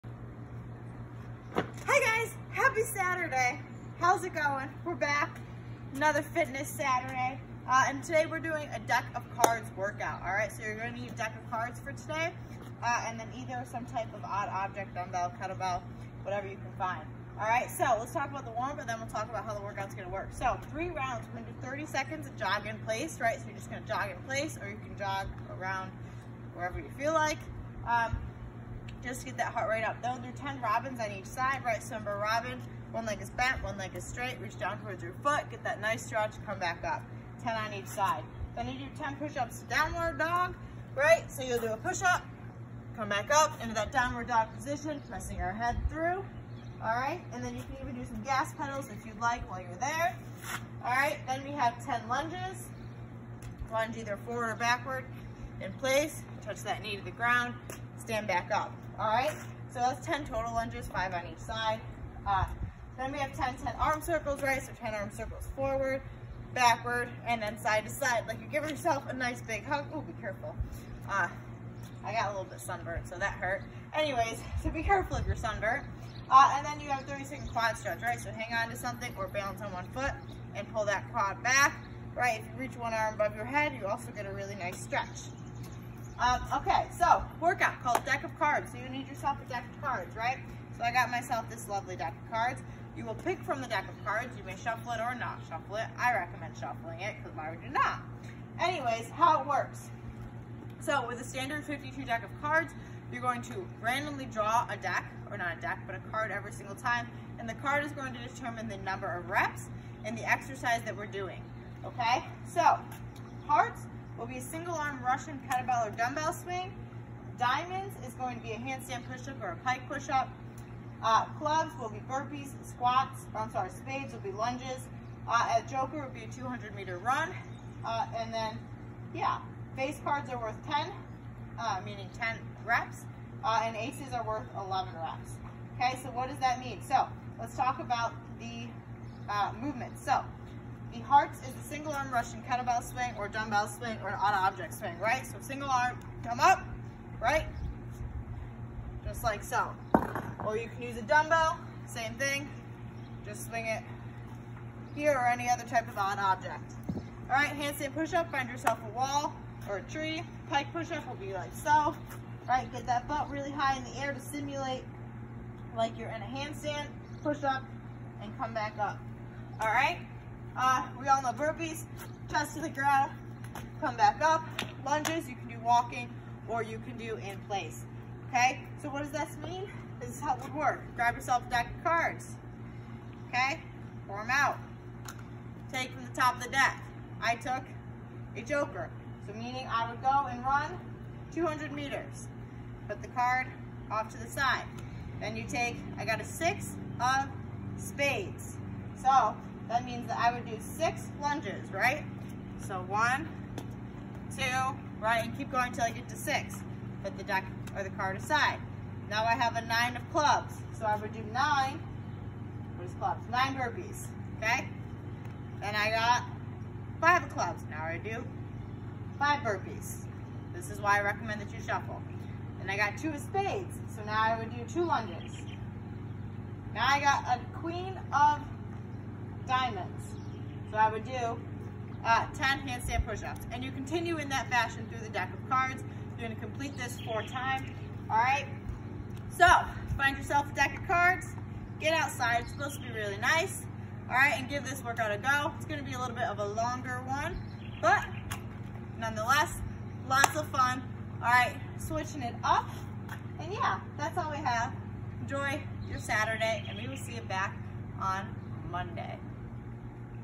SHCF At Home WOD 5.23.20 “Deck of Death”
Warm-Up:
3 rounds
:30 jog in place
10 robins each side
10 push-up to down dog
10 Lunges
10,10,10 arm circles
:30 Quad stretch

MetCon: “Deck of Death”
Complete the Deck for Time: Draw cards from a standard deck of 52 playing cards, each card determines which exercise to do and the number of reps

Hearts: Single-arm Russian Kettlebell/ Dumbbell swing
Diamonds: Handstand push-up/pike push-up
Clubs: Burpees
Spades: Lunges

Joker: 200m run
*Ace= 11 reps
*Face Cards= 10 reps